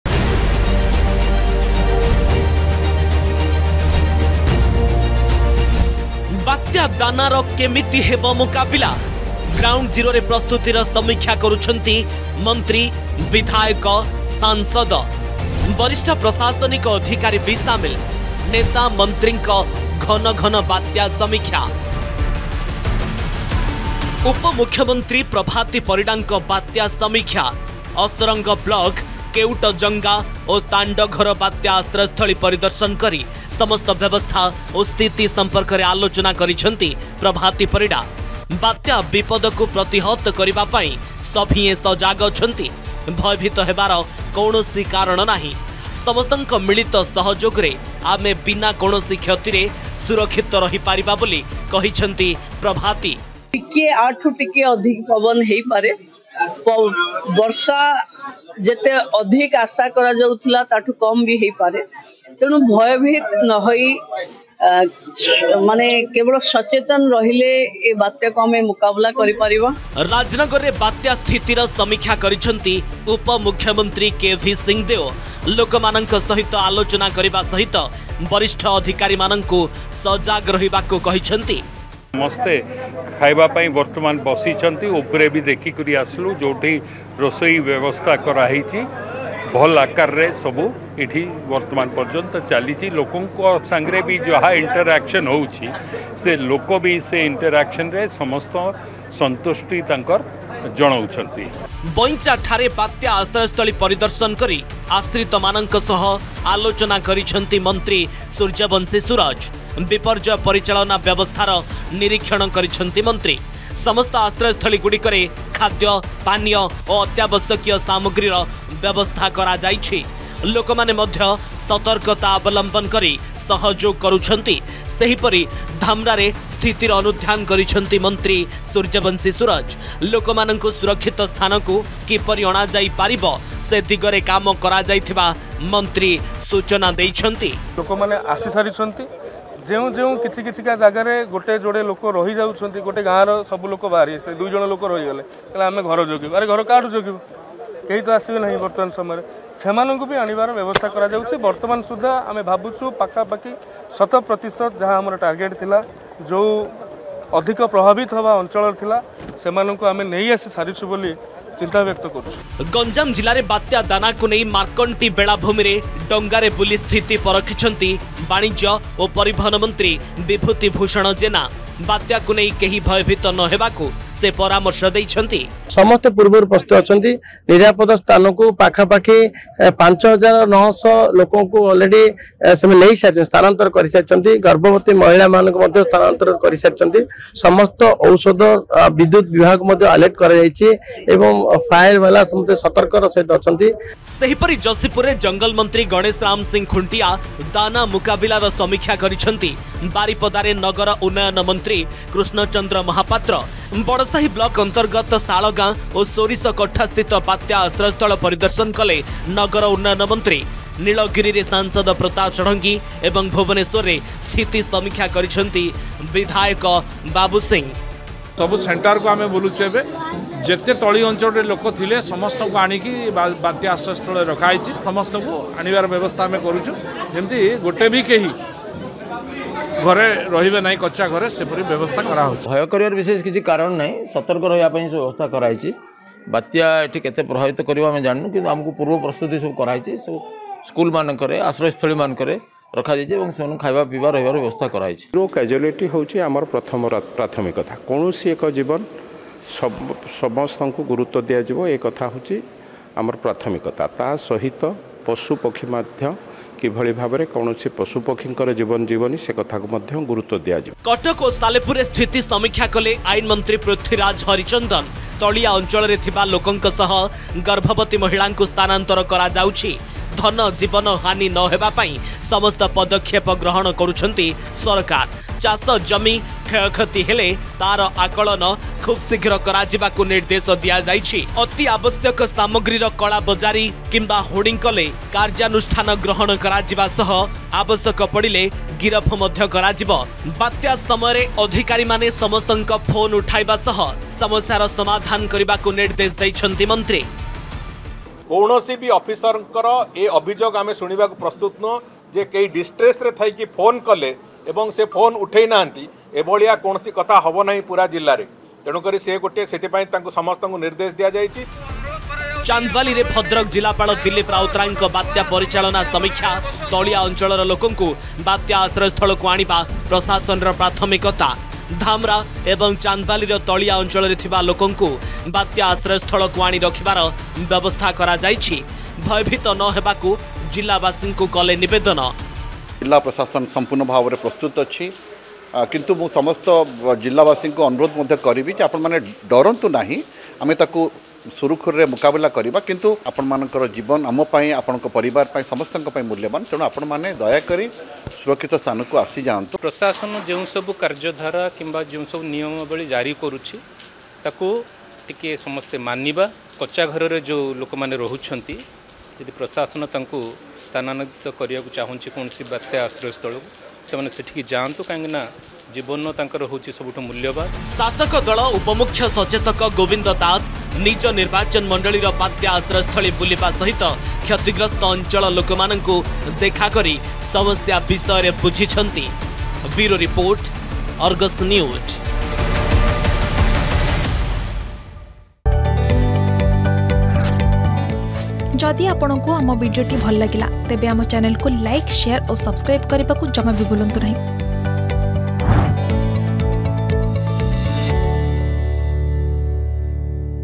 ନେତା ମନ୍ତ୍ରୀଙ୍କ ଘନ ଘନ ବାତ୍ୟା ସମୀକ୍ଷା | Dana Cyclone Update | Odisha Govt
ବାତ୍ୟା ଦାନାର ମୁକାବିଲା ପାଇଁ ଗ୍ରାଉଣ୍ଡ ଜିରୋରେ ପ୍ରସ୍ତୁତିର ସମୀକ୍ଷା କରୁଛନ୍ତି ସାଂସଦ, ମନ୍ତ୍ରୀ, ବିଧାୟକ, ନେତା । ବରିଷ୍ଠ ପ୍ରଶାସନିକ ଅଧିକାରୀ ବି ସ୍ଥିତିର ସମୀକ୍ଷା କରୁଛନ୍ତି ।